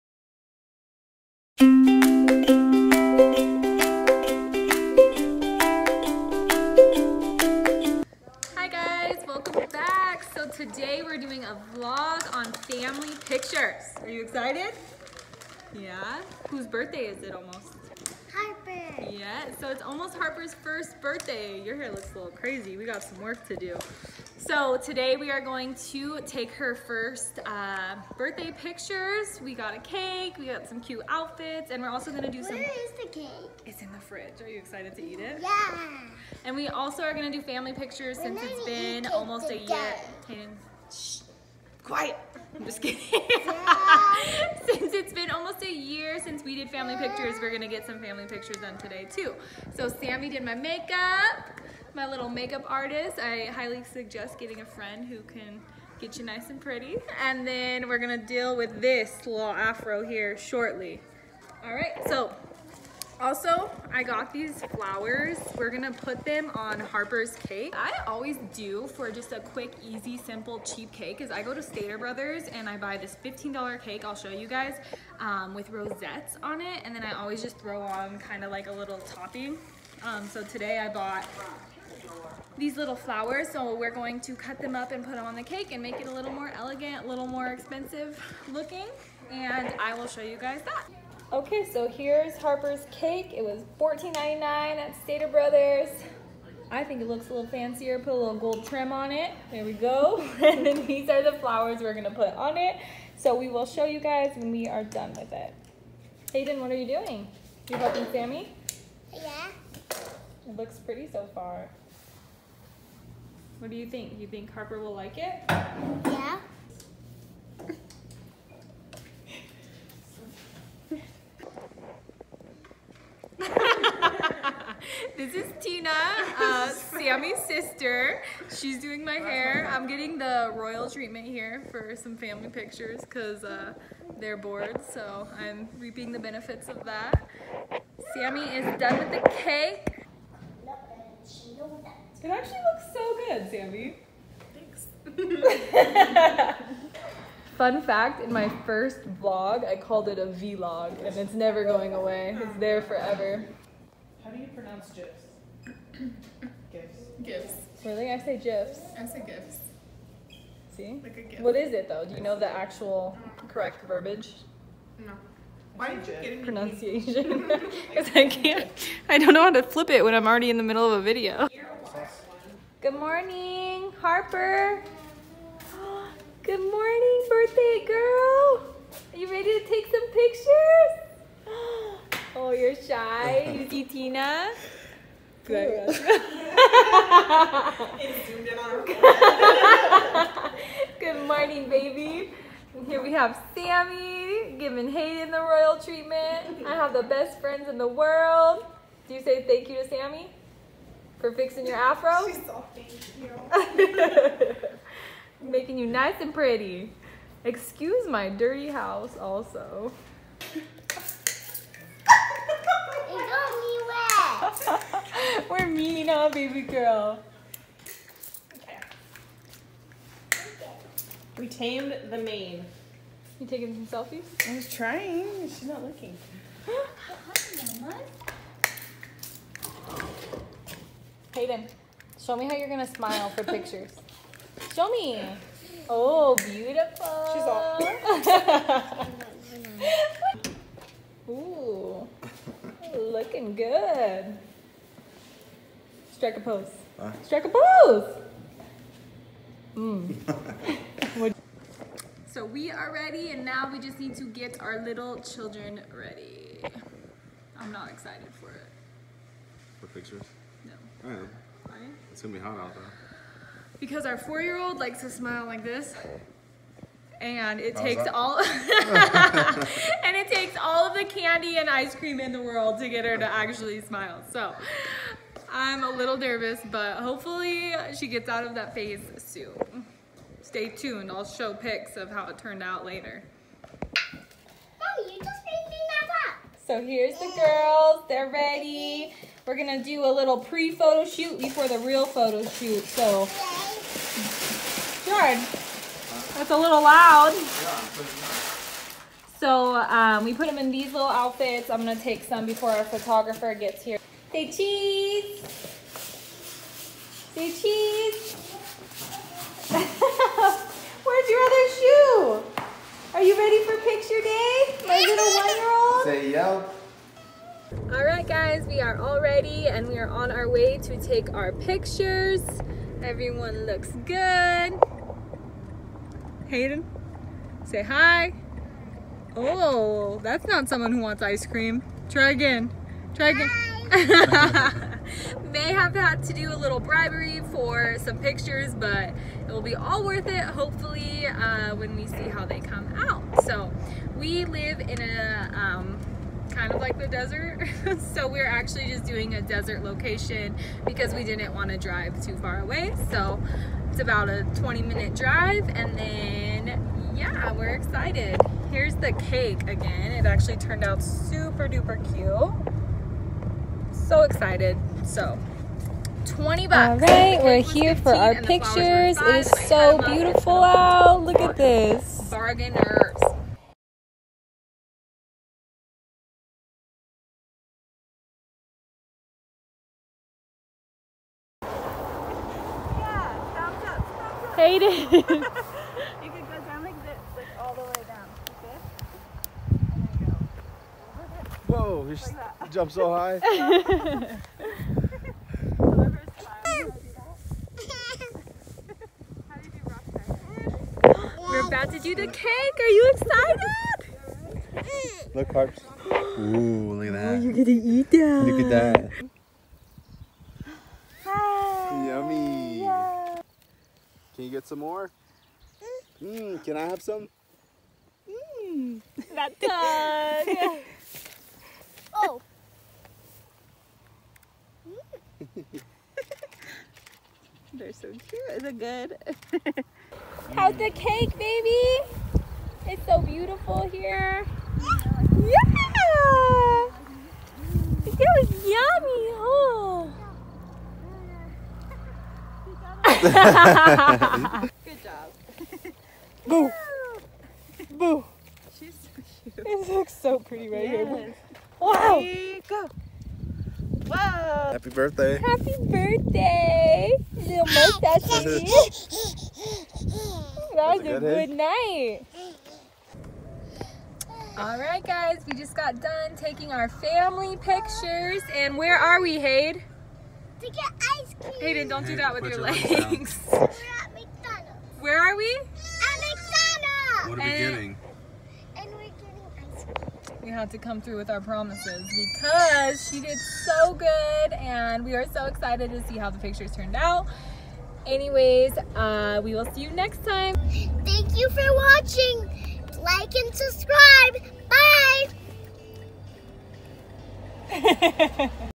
HARPER’S FIRST BDAY PICS. FAMILY PHOTOSHOOT
Today we went to the desert and took pictures for Harper’s first birthday. Watch us struggle getting two kids to cooperate 😂